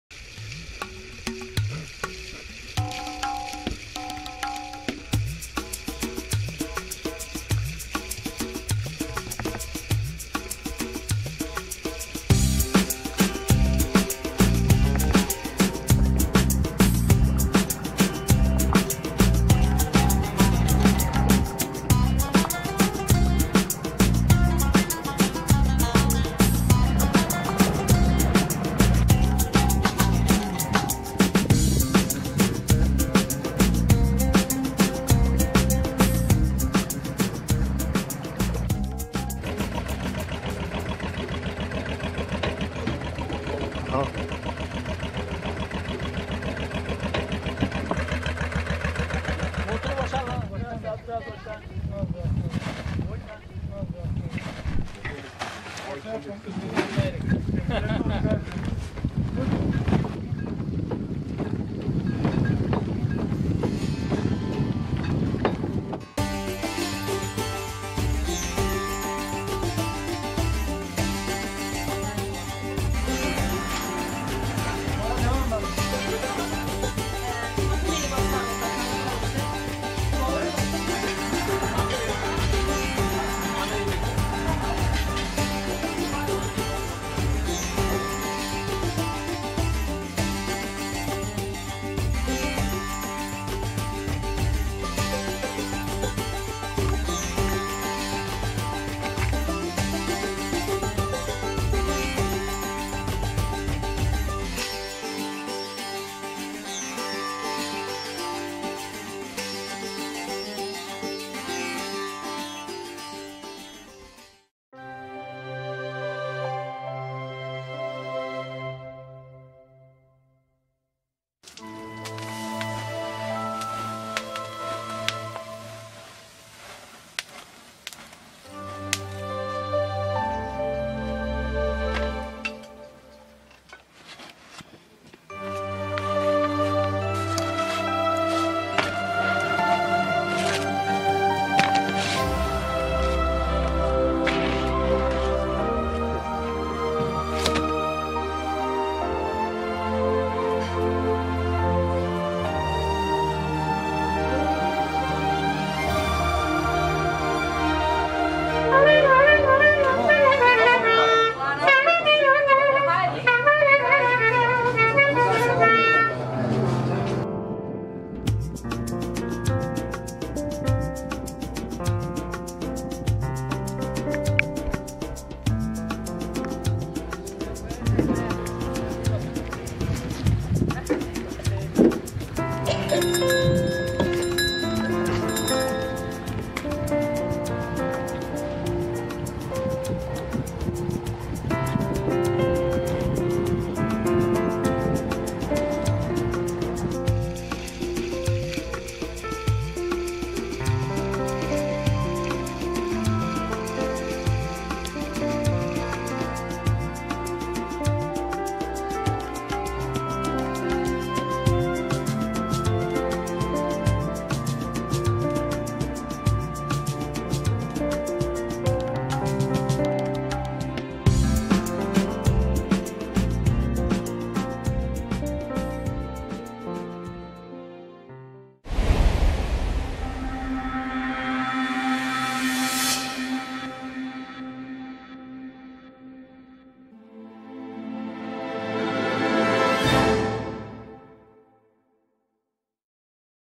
Sailors Paradise Koca Bahce Marmaris Turkey
Sailors Paradise at Koca Bahce on E 36° 41,88' N 028 00,48' is a beautyful and nice restaurant with berth for 20+ yachts on  jetty and mooring. The owner family is very friendly and they serve the best food in the area. Aleways worth to stay one or more nights.